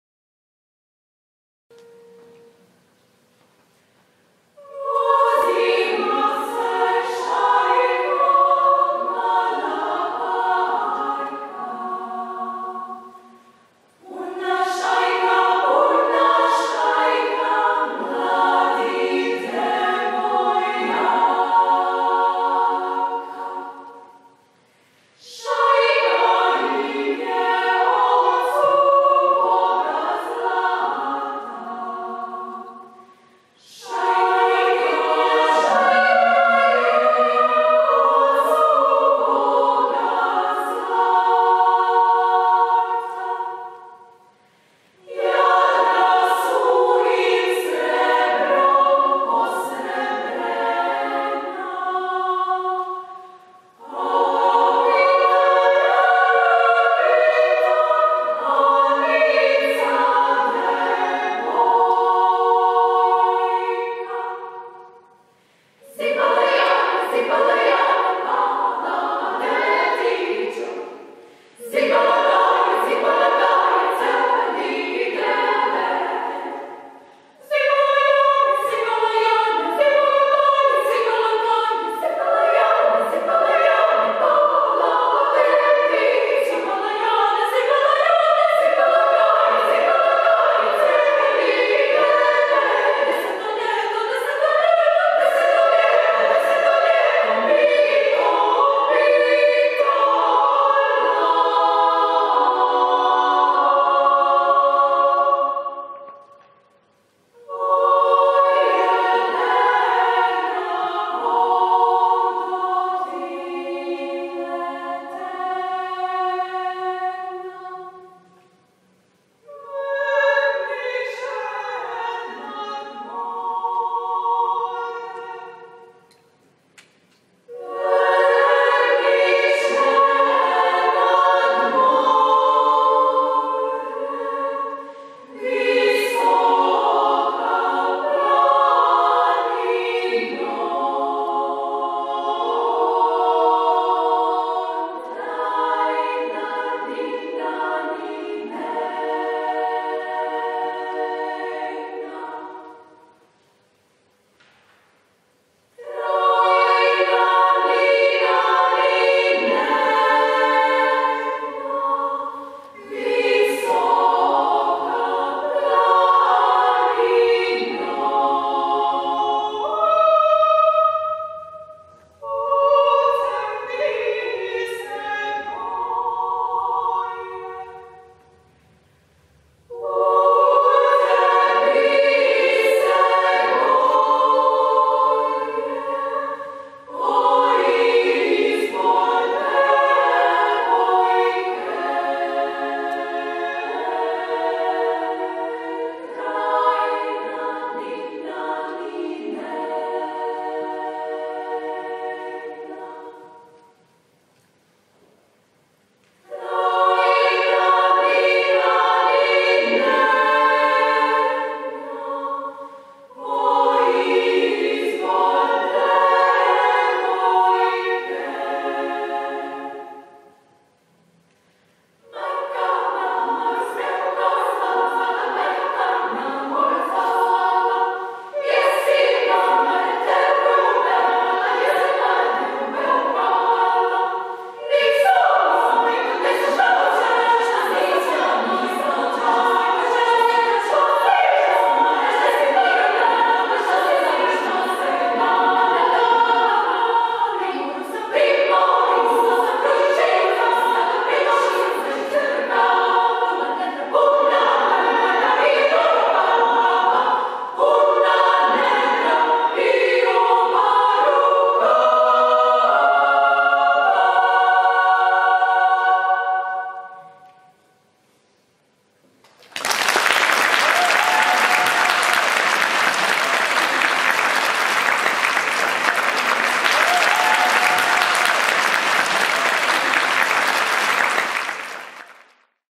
Vokalni Ensemble Brevis in Sydney 2017
World Projects South Pacific is once again conducting the Australian International Music Festival in Sydney. Vokalni Ensemble Brevis from Osijek, Croatia is one of the many choirs performing, winning praise for their beautiful singing. Here is a  one example of their superb voices.
Video by Seen Australia.